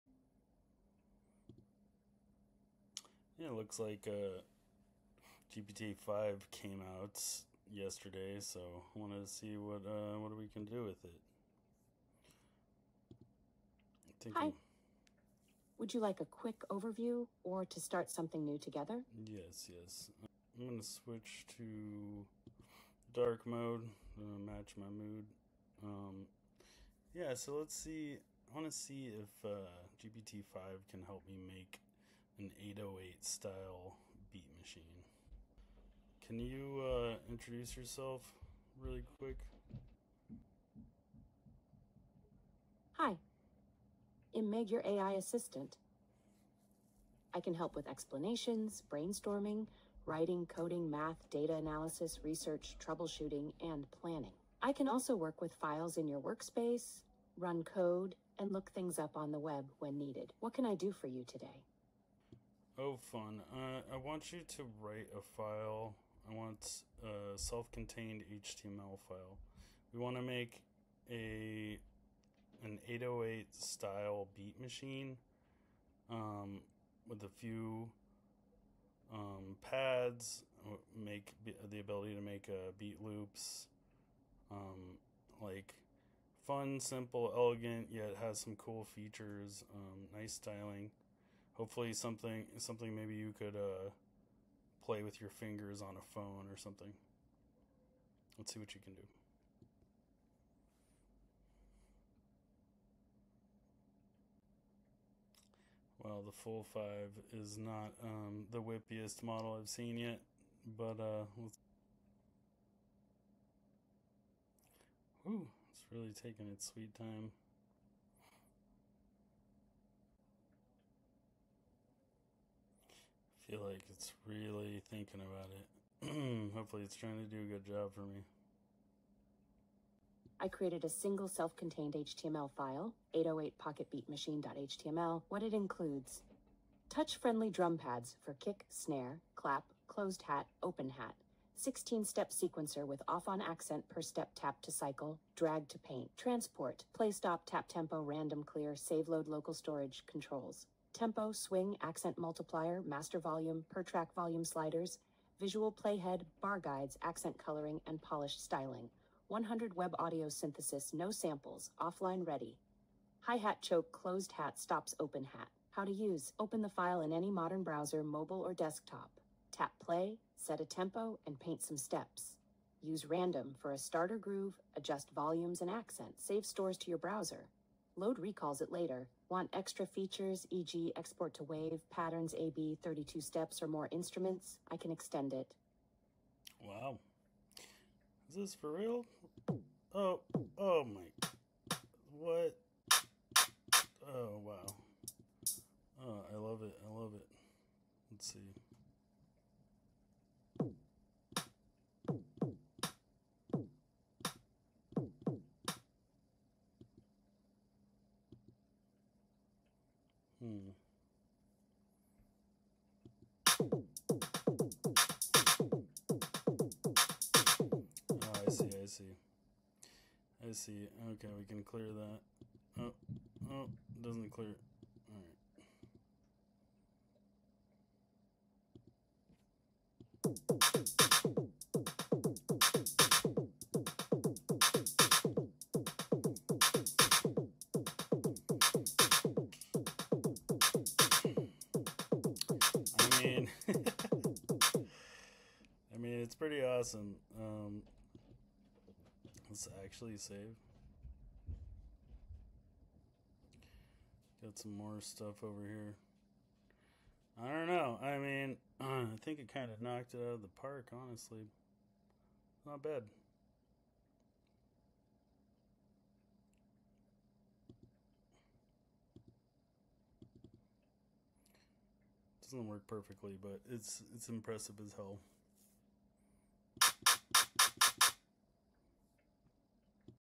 Creating an 808 Style Beat Machine with GPT 5 🎶
Lil vibe code sesh with gpt 5 and mage lab

magelab.ai